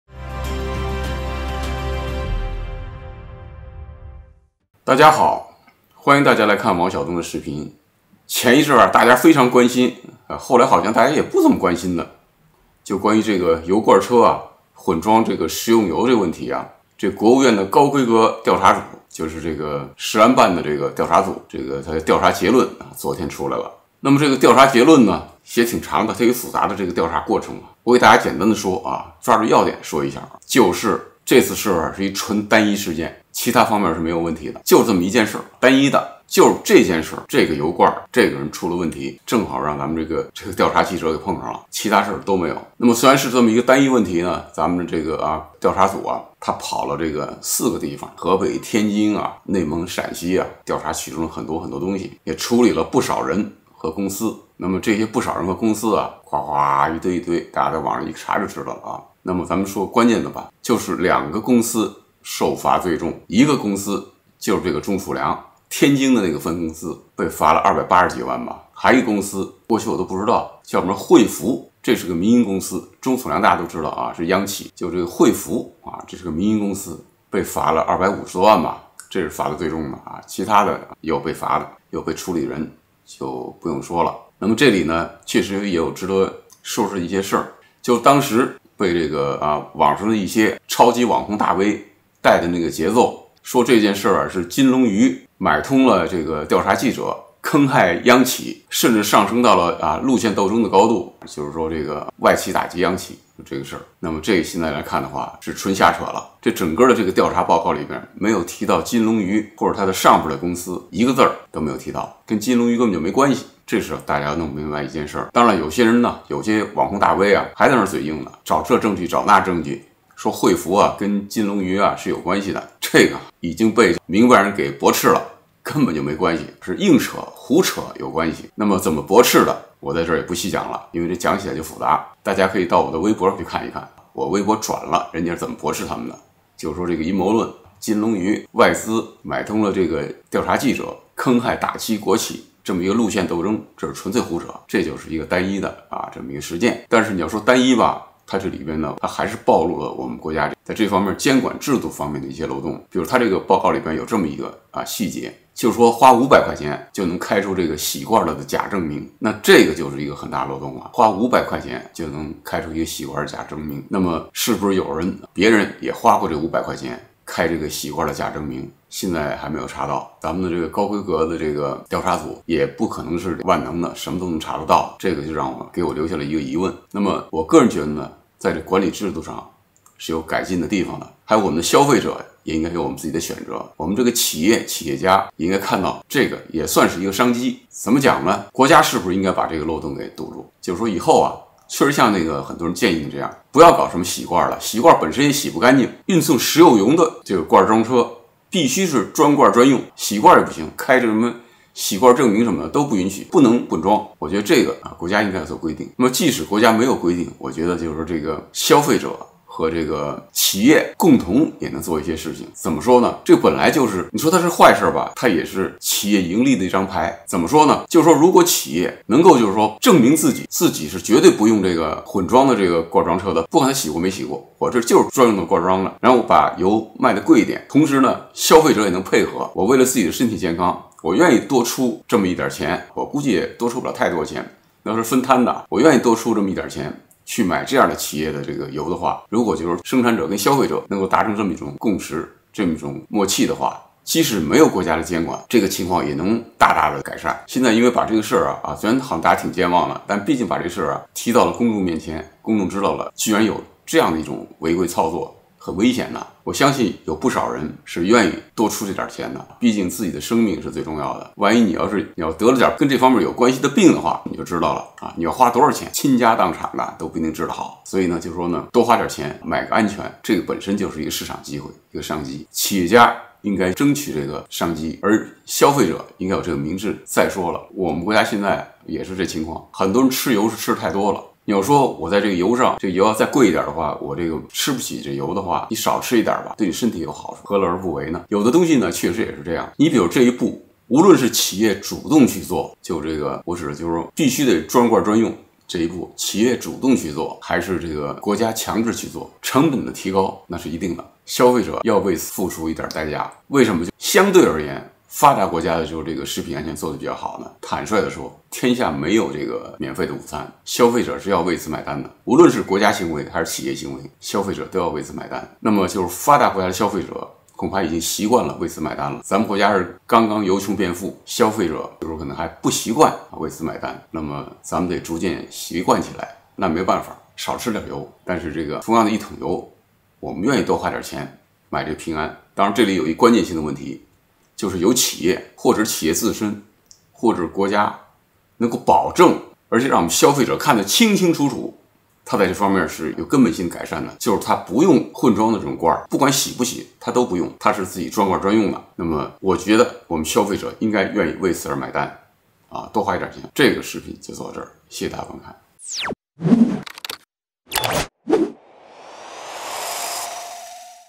國家對食用油油罐混裝調查結論出來了，跟外企陰謀沒關係
大家好，歡迎大家來看王小東的視頻